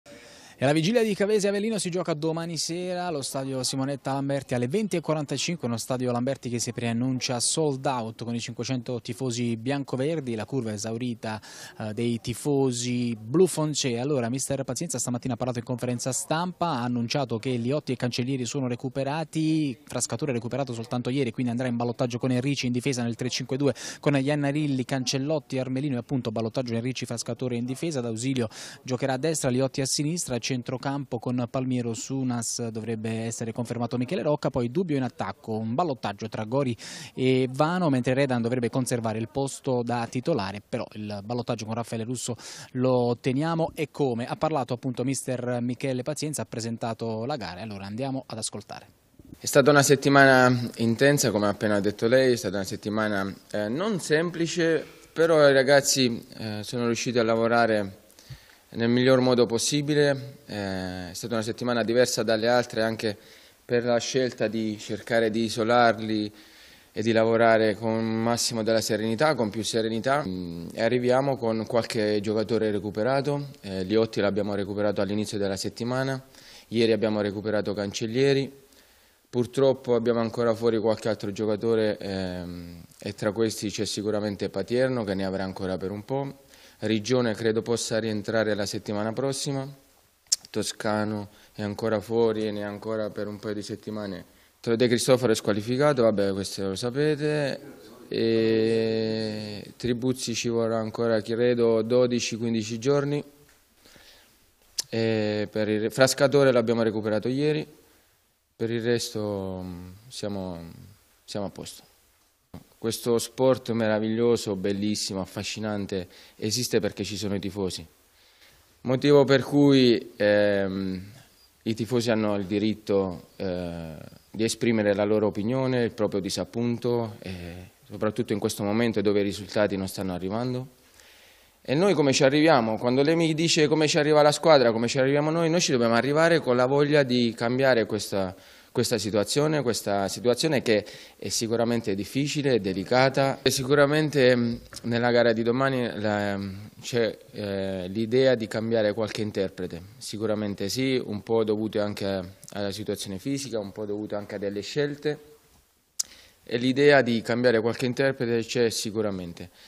Pazienza recupera Liotti. Due dubbi di formazione per l'Avellino
Le ultime sull'Avellino e la probabile formazione biancoverde